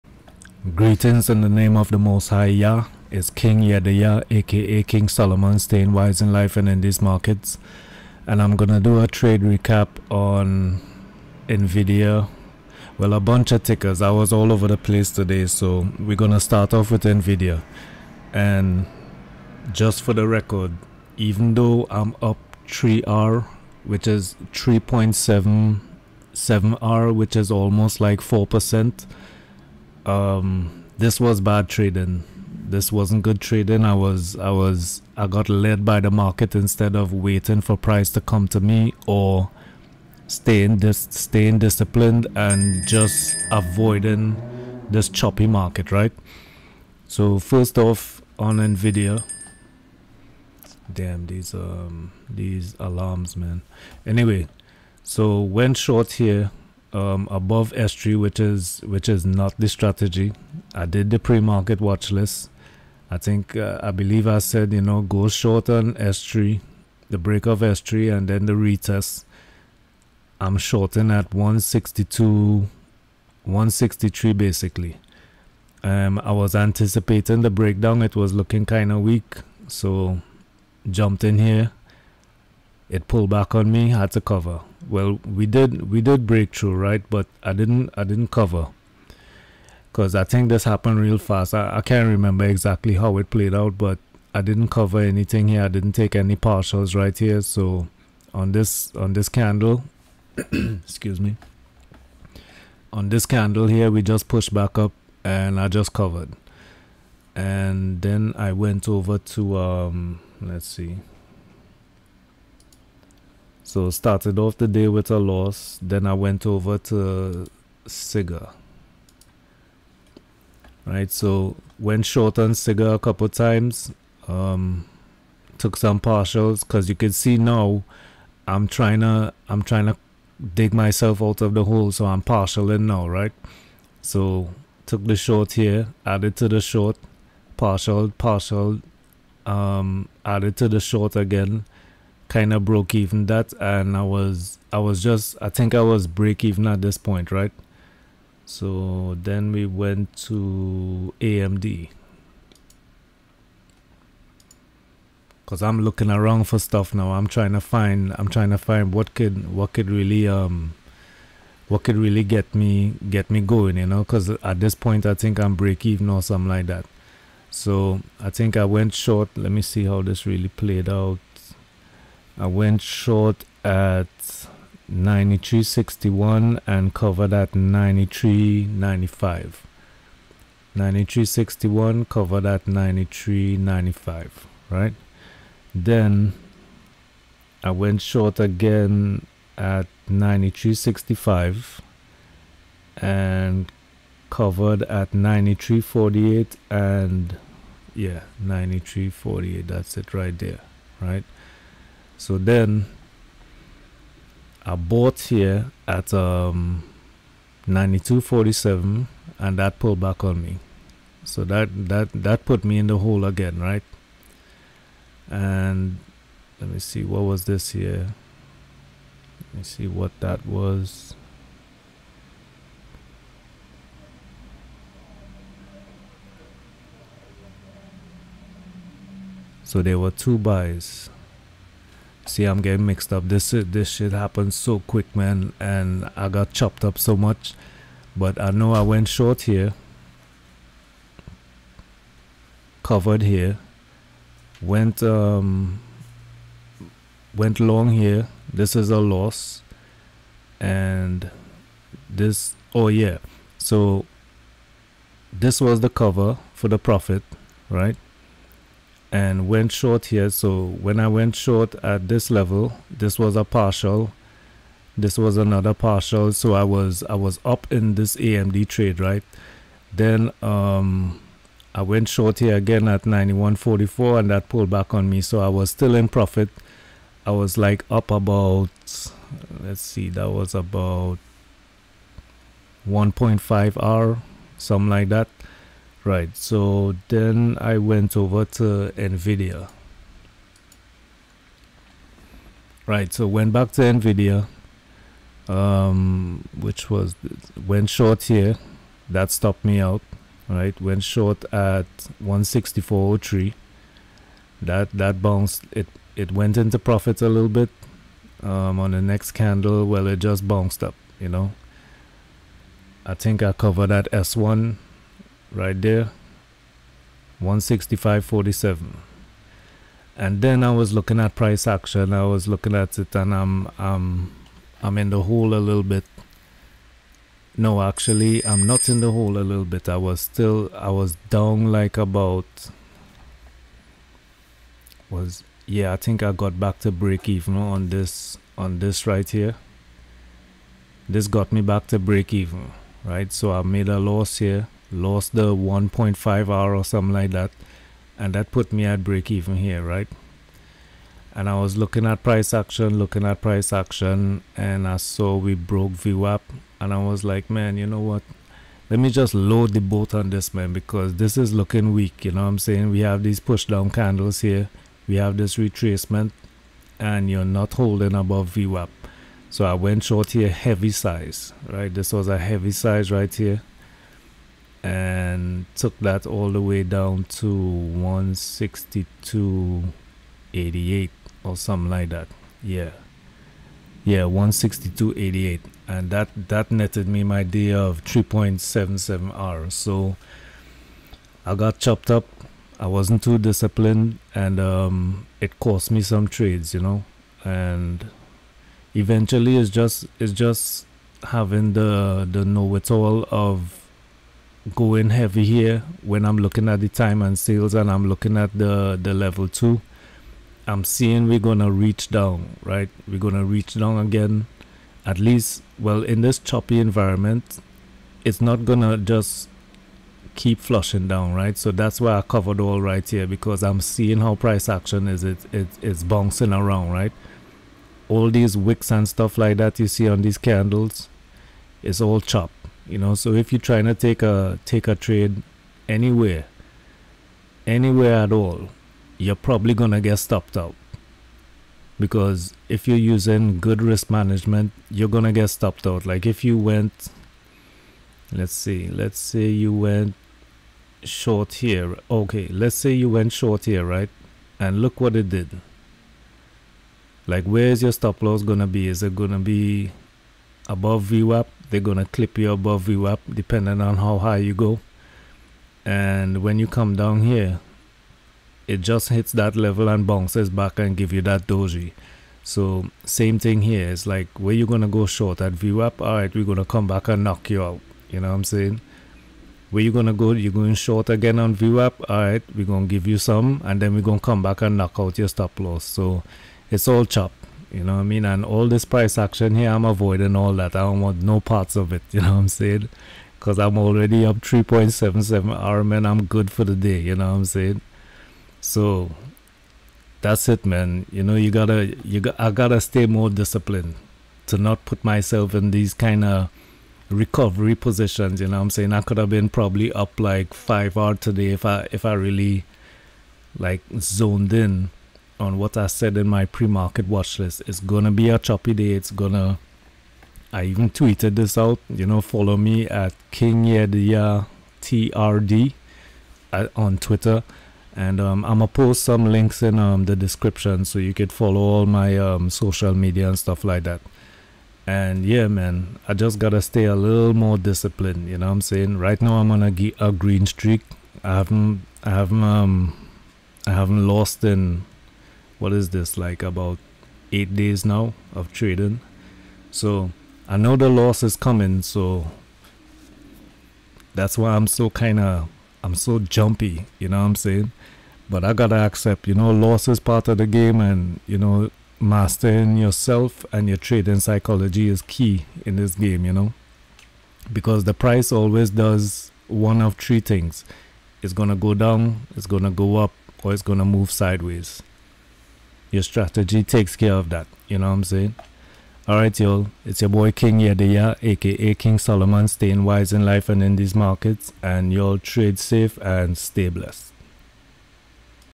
TRADE RECAP MONDAY 23RD MAY 2022 NVDA SIGA AMD
Trading a choppy market and getting swayed to and fro. Made some, gave some back and then focused on the Level@ and Time and Sales on NVDA and went short heavy share size for the 3.77 % gain. Done for the day